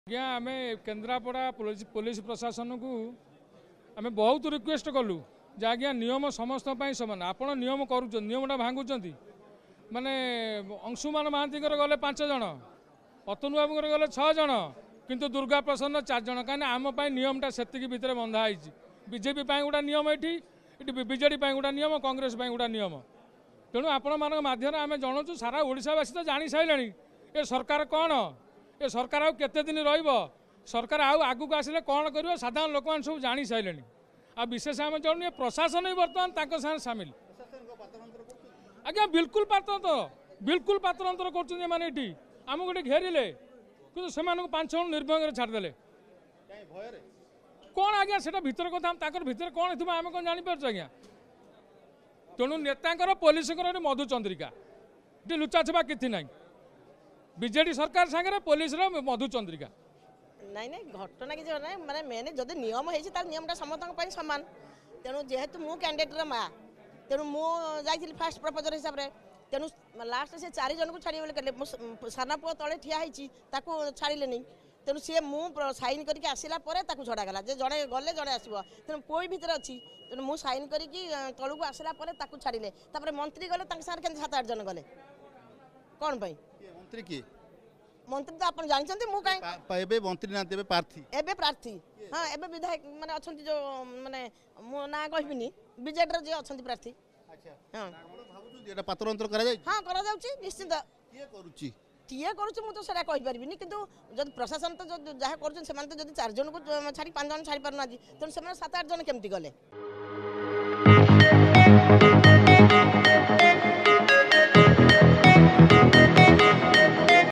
‘ଆମକୁ ଘେରିଲେ, ସେମାନଙ୍କୁ ଛାଡିଦେଲେ’ | Election 2024 | Kendrapada Police | BJD | BJP | Odisha Reporter
ଆମେ କେନ୍ଦ୍ରାପଡା ପୋଲିସ ପ୍ରଶାସନକୁ ବହୁତ ଅନୁରୋଧ କଲୁ। ନିୟମ ସମସ୍ତଙ୍କ ପାଇଁ ସମାନ, ଆପଣ ନିୟମ କରୁଛନ୍ତି ପୁଣି ନିୟମ ଭାଙ୍ଗୁଛନ୍ତି ବୋଲି କହିଛନ୍ତି ଜଣେ ବିଜେପି ସମର୍ଥକ।